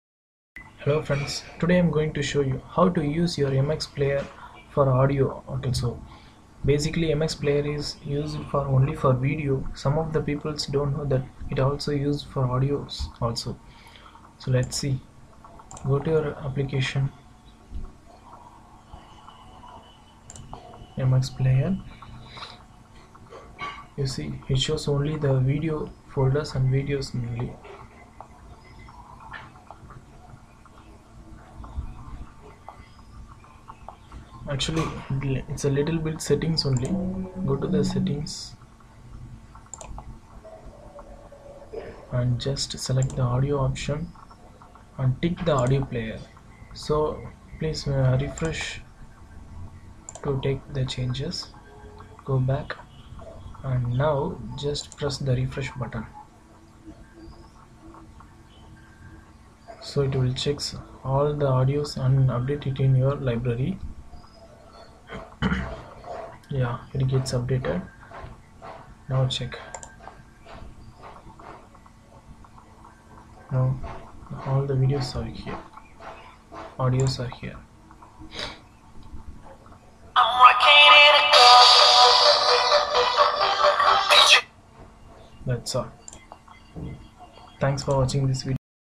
Use MX player for Audio | Android
This video for Playing Audio Files in MX player, because most of the people dont know it supports audio files...